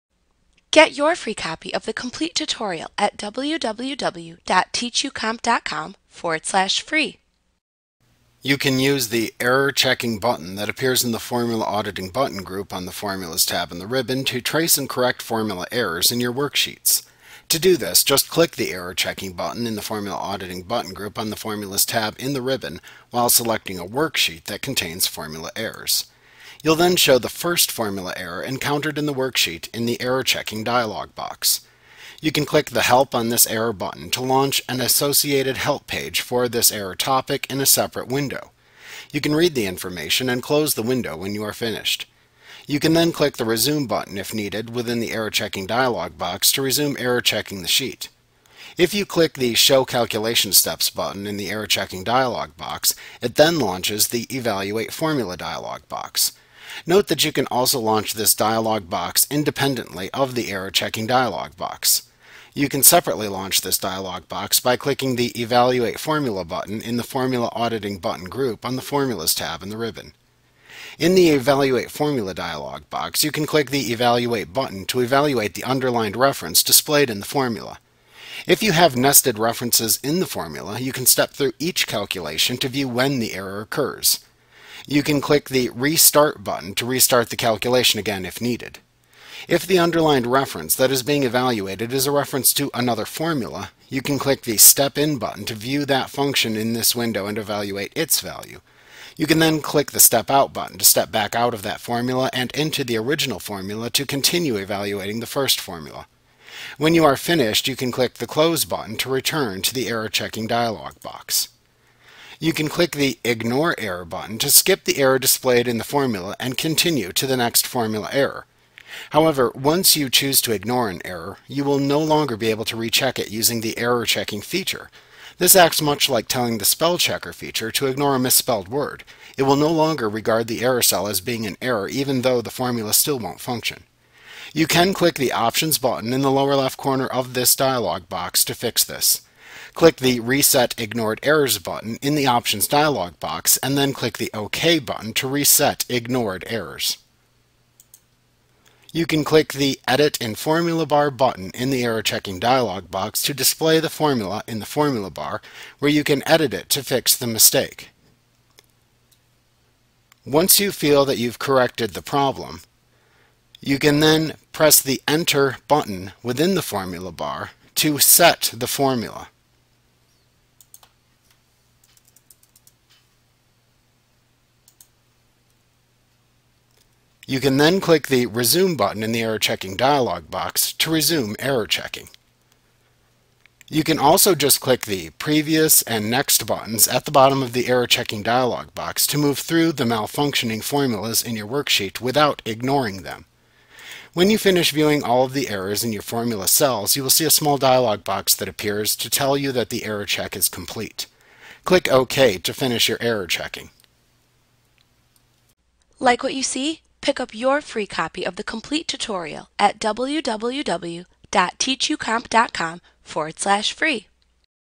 Excel 2016 Tutorial Error Checking Microsoft Training Lesson
Learn how to use error checkings in Microsoft Excel A clip from Mastering Excel Made Easy v. 2016. - the most comprehensive Excel tutorial available. Visit us today!